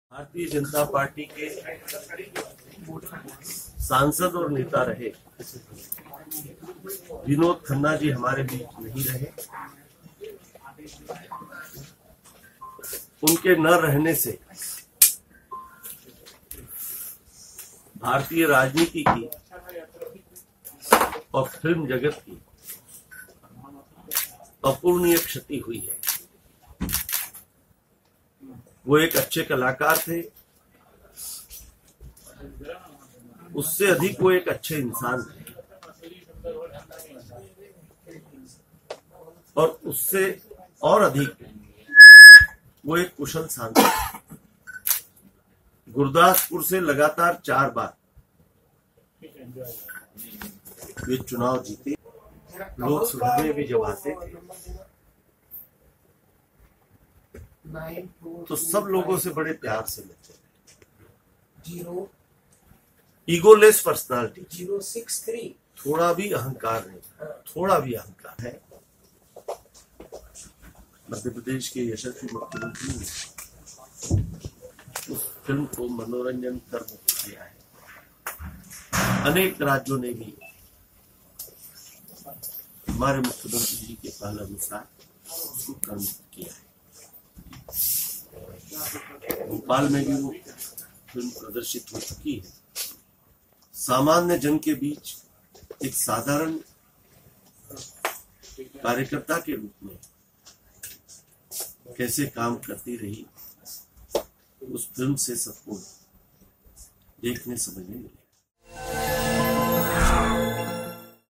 vinod khanna | MInister said about actor vinod khanna
MInister  Nand kumar chauhan said about  actor vinod khanna